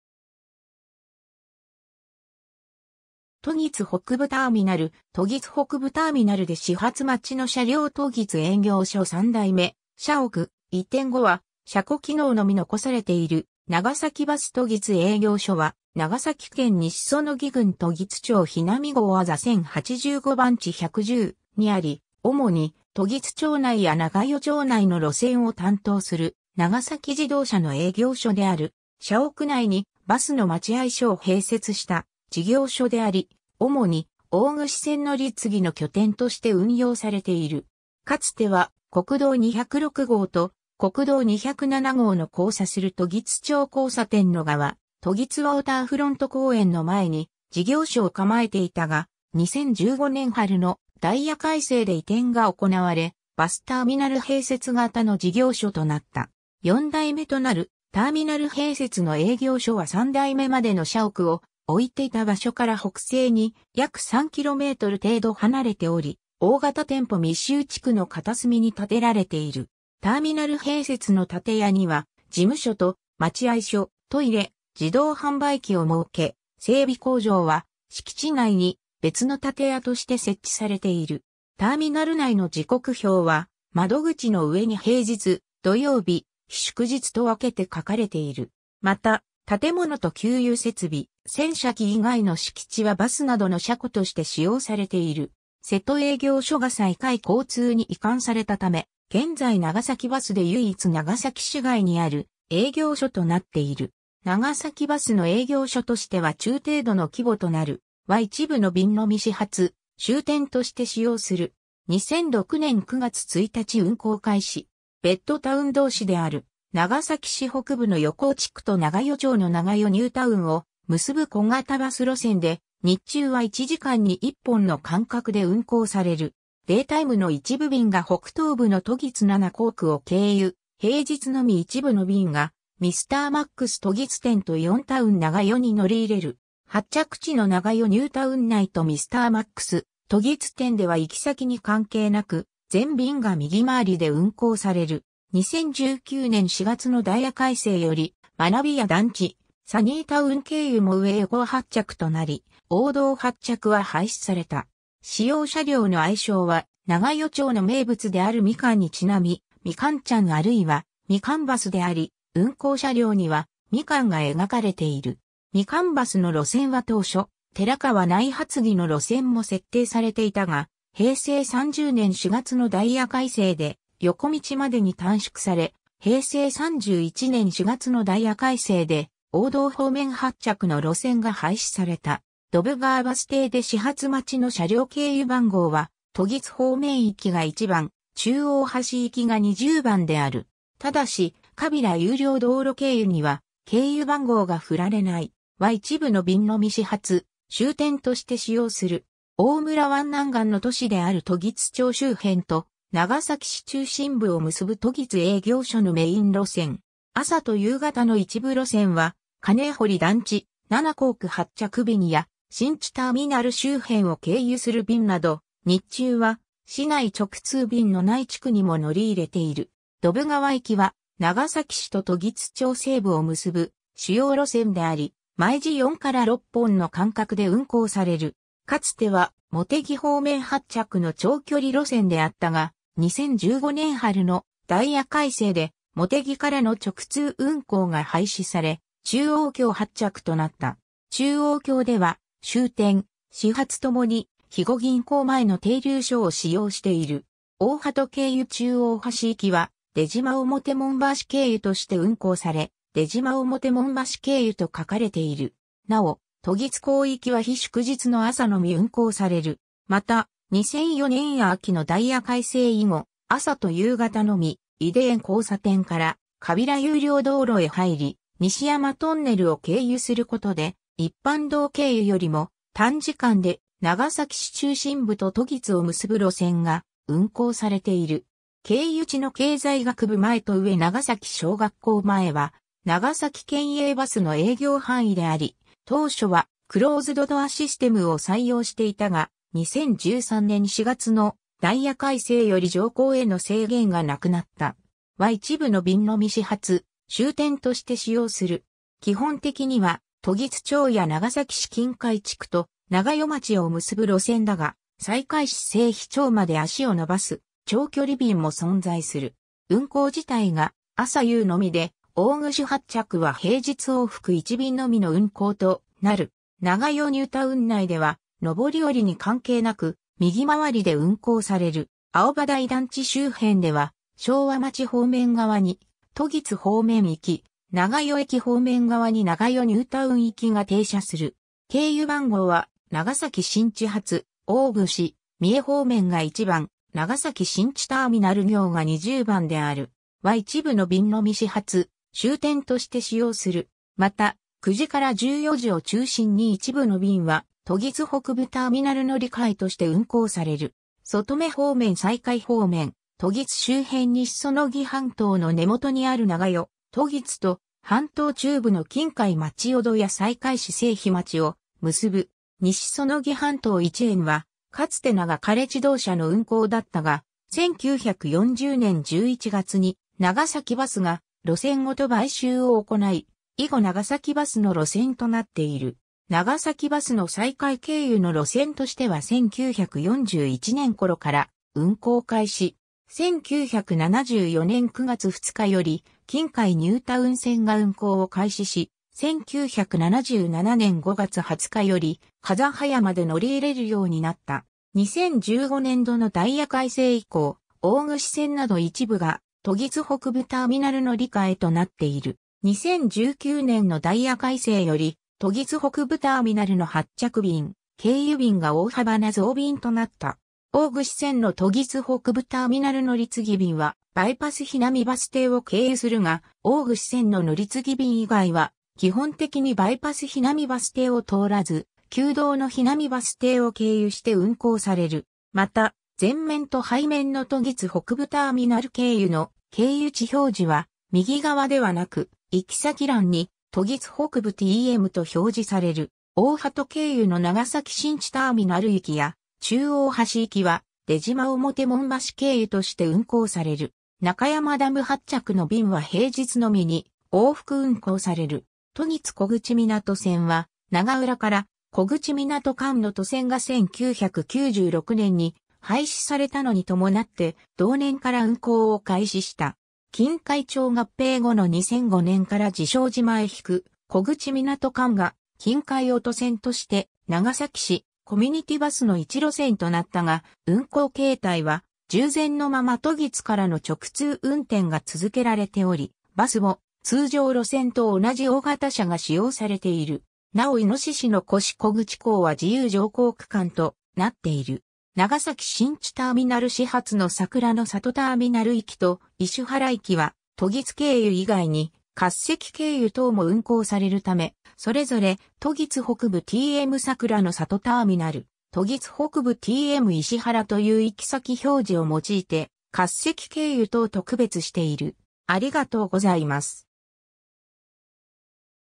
長崎バス時津営業所
時津北部ターミナル（時津営業所四代目社屋） 時津北部ターミナルで始発待ちの車両 時津営業所三代目社屋、移転後は車庫機能のみ残されている 長崎バス時津営業所（ながさきバスとぎつえいぎょうしょ）は長崎県西彼杵郡時津町日並郷字1085番地110にあり、主に時津町内や長与町内の路線を担当する長崎自動車の営業所である。
 社屋内にバスの待合所を併設した事業所であり、主に大串線乗り継ぎの拠点として運用されている。
かつては国道206号と国道207号の交差する時津町交差点の側、時津ウォーターフロント公園の前に事業所を構えていたが、2015年春のダイヤ改正で移転が行われ、バスターミナル併設型の事業所となった。
四代目となるターミナル併設の営業所は三代目までの社屋を置いていた場所から北西に約3km程度離れており、大型店舗密集地区の片隅に建てられている。
ターミナル併設の建屋には事務所と待合所、トイレ、自動販売機を設け、整備工場は敷地内に別の建屋として設置されている。
ターミナル内の時刻表は窓口の上に平日、土曜日、日祝日と分けて書かれている。
また、建物と給油設備、洗車機以外の敷地はバスなどの車庫として使用されている。
 瀬戸営業所（西海市）がさいかい交通に移管されたため、現在長崎バスで唯一長崎市外にある営業所となっている。
長崎バスの営業所としては中程度の規模となる。
 ※は一部の便のみ始発・終点として使用する。
 2006年（平成18年）9月1日運行開始。
ベッドタウン同士である長崎市北部の横尾地区と長与町の長与ニュータウンを結ぶ小型バス路線で、日中は1時間に1本の間隔で運行される。
データイムの一部便が北東部の時津七工区を経由、平日のみ一部の便がミスターマックス時津店とイオンタウン長与に乗り入れる。
発着地の長与ニュータウン内とミスターマックス時津店では行き先に関係なく全便が右回りで運行される。
2019年4月のダイヤ改正より、まなび野団地、サニータウン経由も上横尾発着となり、横道発着は廃止された。
使用車両の愛称は長与町の名物であるミカンにちなみ「みかんちゃん」或いは「みかんバス」であり、運行車両にはミカンが描かれている。
 みかんバスの路線は当初、寺川内発着の路線も設定されていたが、平成30年（2018年）4月のダイヤ改正で横道までに短縮され、平成31年4月のダイヤ改正で横道方面発着の路線が廃止された。
 溝川バス停で始発待ちの車両 経由番号は時津方面行が1番、中央橋行きが20番である。
ただし川平有料道路経由には経由番号が振られない。
 大村湾南岸の都市である時津町周辺と長崎市中心部を結ぶ時津営業所のメイン路線。
朝と夕方の一部路線は金堀団地・七工区発着便や新地ターミナル周辺を経由する便など、日中は市内直通便のない地区にも乗り入れている。
溝川行きは長崎市と時津町西部を結ぶ主要路線であり、毎時4〜6本の間隔で運行される。
かつては茂木方面発着の長距離路線であったが、2015年春のダイヤ改正で茂木からの直通運行が廃止され、中央橋発着となった。
中央橋では終点・始発共に肥後銀行前の停留所を使用している。
大波止経由中央橋行きは出島表門橋経由として運行され、出島表門橋経由と書かれている。
なお、時津港行きは日祝日の朝のみ運行される。
 また、2004年秋のダイヤ改正以後、朝と夕方のみ井手園交差点から川平有料道路へ入り、西山トンネルを経由することで一般道経由よりも短時間で長崎市中心部と時津を結ぶ路線（通称川平バイパス線）が運行されている。
経由地の経済学部前（長崎大学片淵キャンパス）と上長崎小学校前（旧：西山郵便局前）は長崎県営バスの営業範囲であり、当初はクローズドドアシステムを採用していたが、2013年4月のダイヤ改正より乗降への制限がなくなった。
 基本的には時津町や長崎市琴海地区と長与町を結ぶ路線だが、西海市西彼町まで足をのばす長距離便も存在する。
運行自体が朝夕のみで大串発着は平日往復1便のみの運行となる。
長与ニュータウン内では、上り下りに関係なく右回りで運行される。
青葉台団地周辺では昭和町方面側に時津方面行き、長与駅方面側に長与ニュータウン行きが停車する。
 経由番号は長崎新地発大串・三重方面が1番、長崎新地ターミナル行が20番である。
また、9時〜14時を中心に一部の...